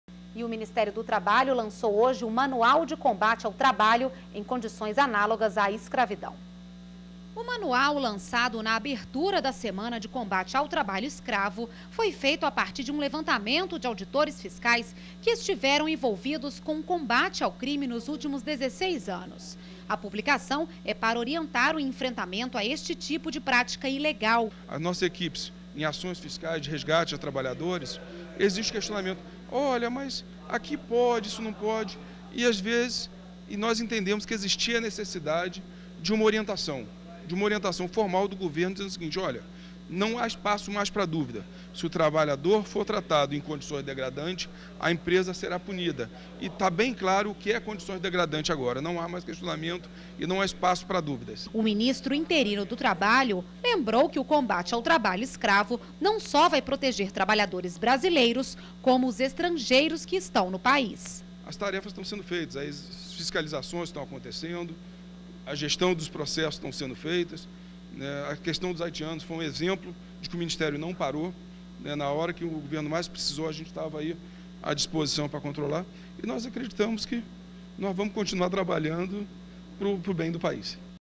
Lançado manual de combate ao trabalho análogo à escravidão
NBR Notícias - 24.01.11: O Ministério do Trabalho lançou nesta terça-feira (24/01), o Manual de Combate ao Trabalho em Condições Análogas à Escravidão.